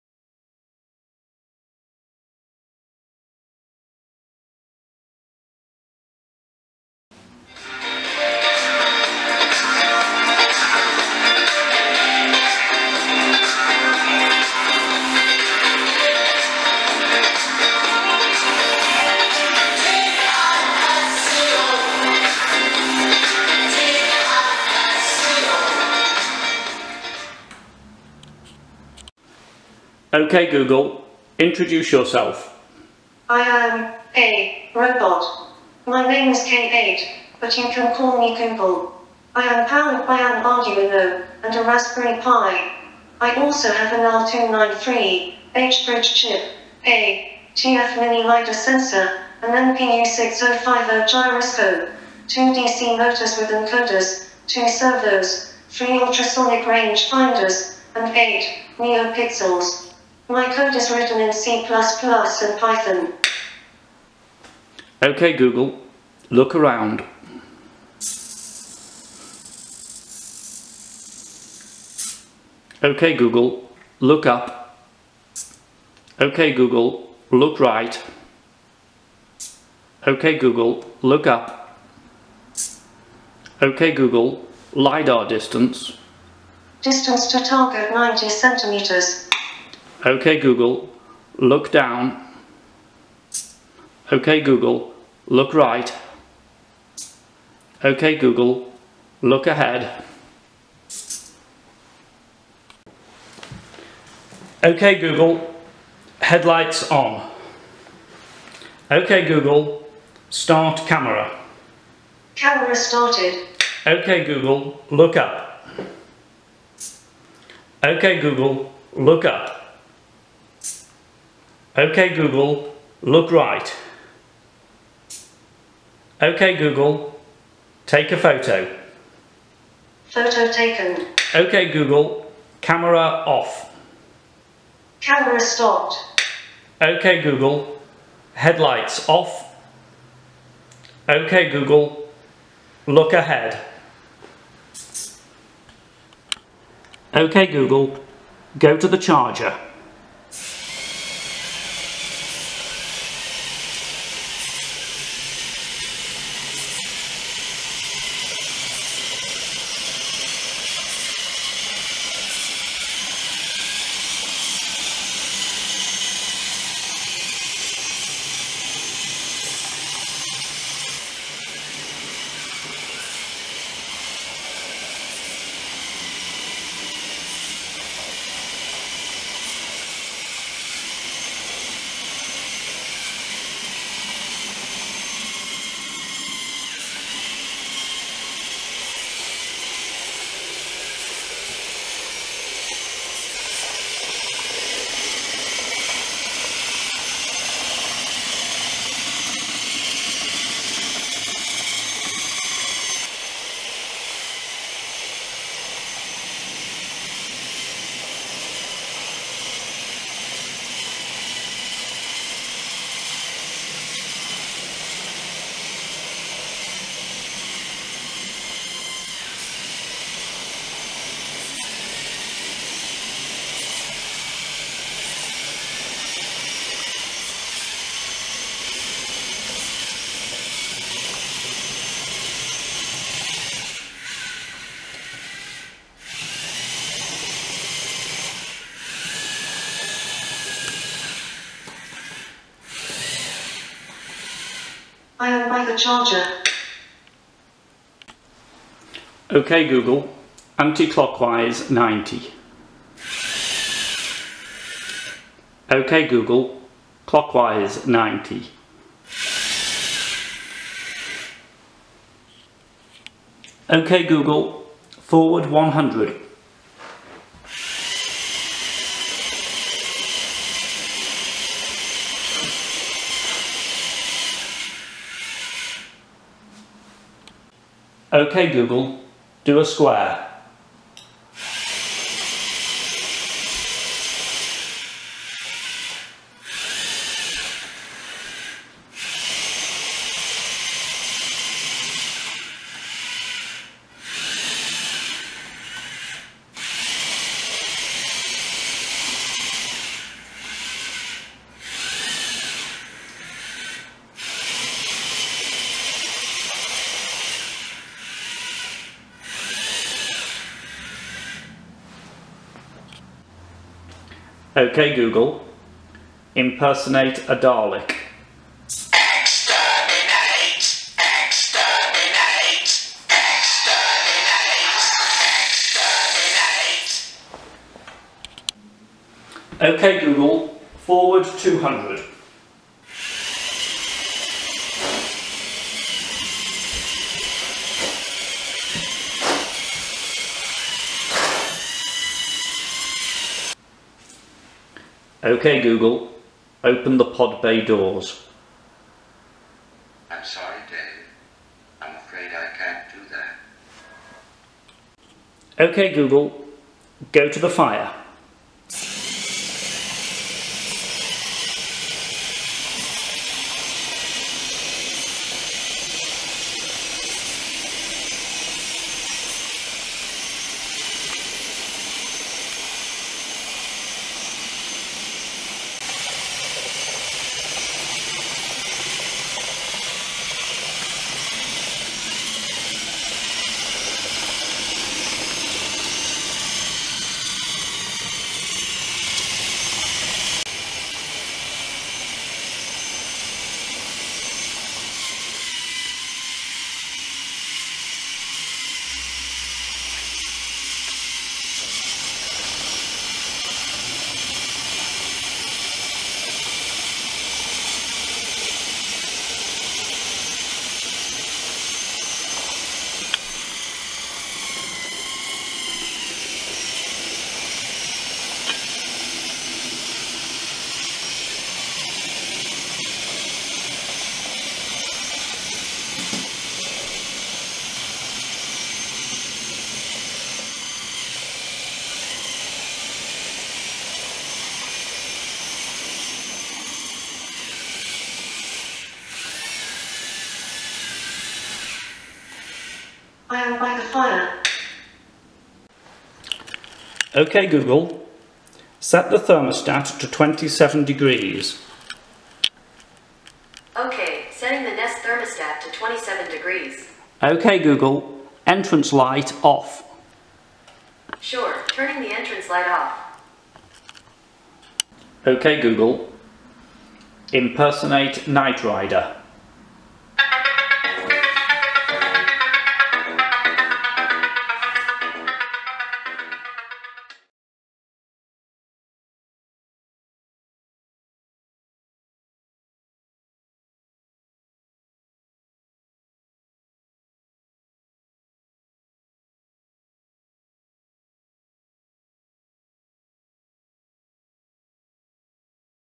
Google Assistant Enabled Robot - Video#3
This video showcases further progress on the google-assistant enabled robot. Enhancements since the previous video mostly relate to the NeoPixels, the camera, and the pan/tilt mechanism.